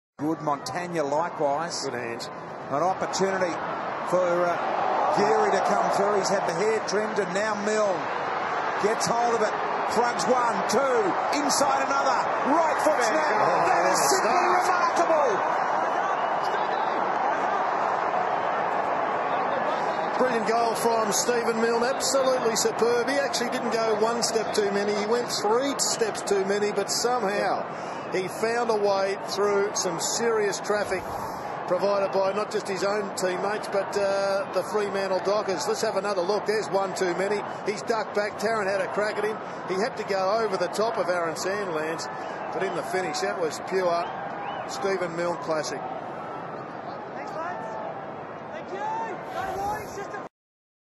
Stephen Milne - Doing what he does best
His first goal in his 200th game - unbelievable!